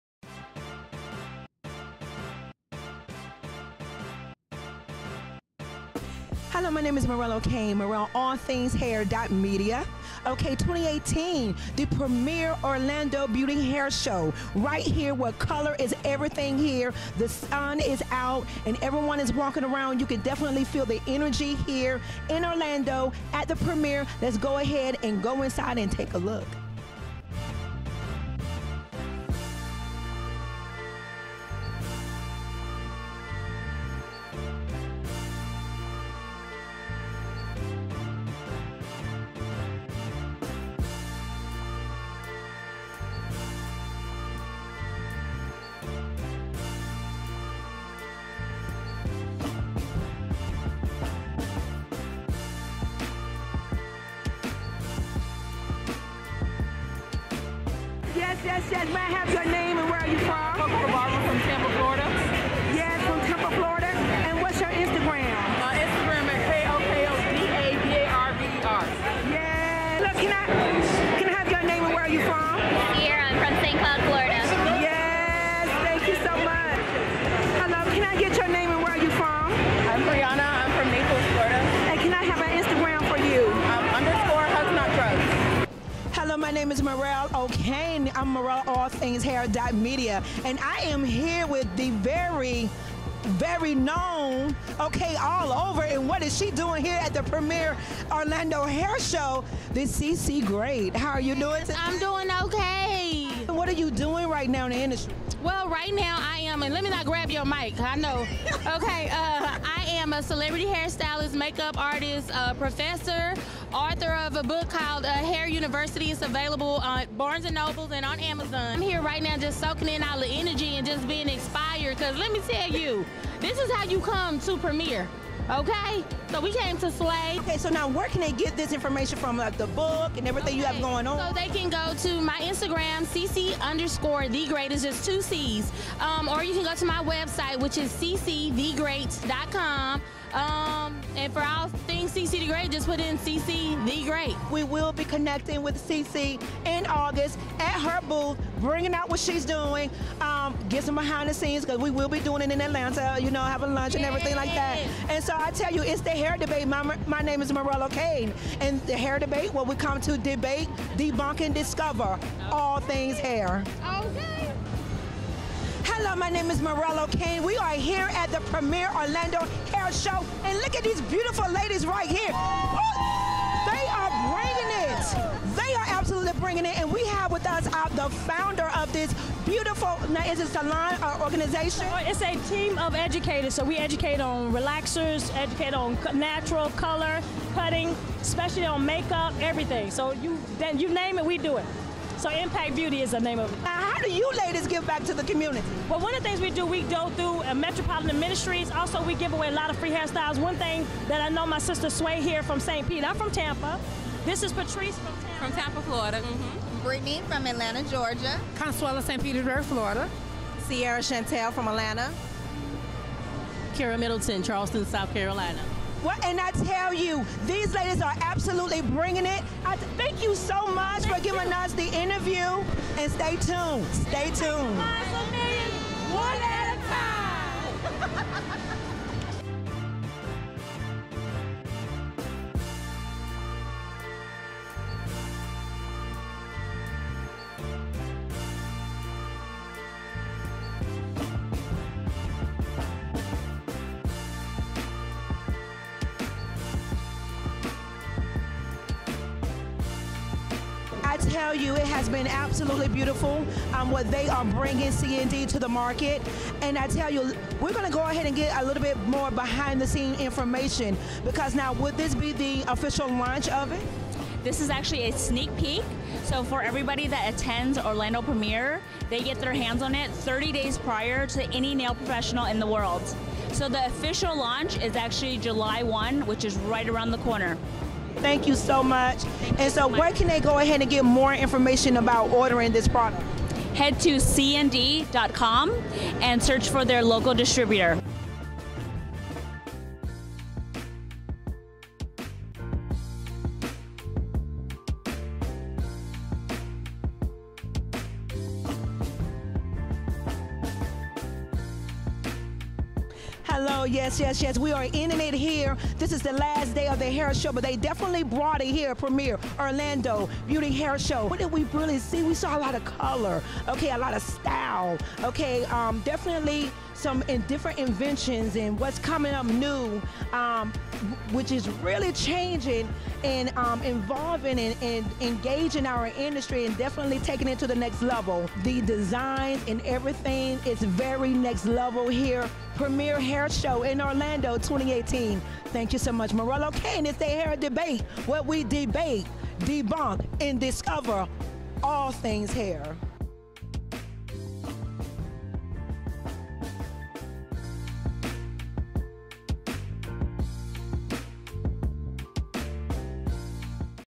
Premiere Orlando 2018 2018 Show Recap
Join us as we explore Premiere Orlando Beauty Show 2018! We have collaborated with T.A.S.B to bring you a few Higlights of This Years beauty Show!